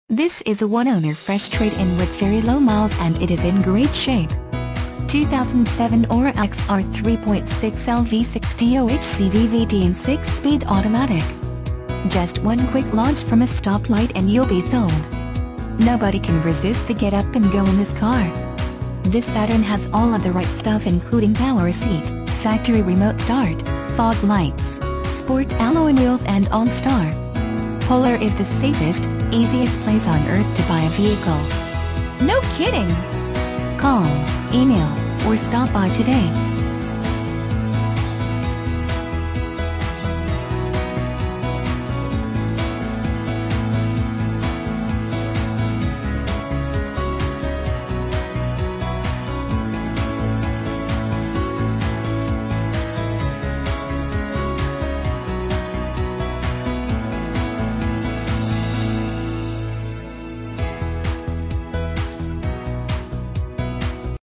2007 SATURN AURA White Bear Lake, MN 16930
2007 SATURN AURA White Bear Lake, MN
Stock #16930

1801 E County Road F

This is a one owner fresh trade-in with very low miles and it is in great shape! 2007 Aura XR 3.6L V6 DOHC VVT and 6-Speed Automatic. Just one quick launch from a stoplight and you'll be SOLD! Nobody can resist the get-up-and-go in this car. This Saturn has all of the right stuff including power seat, factory remote start, fog lights, sport alloy wheels and OnStar. Polar is the safest, easiest place on earth to buy a vehicle...no kidding! Call, e-mail, or stop by today!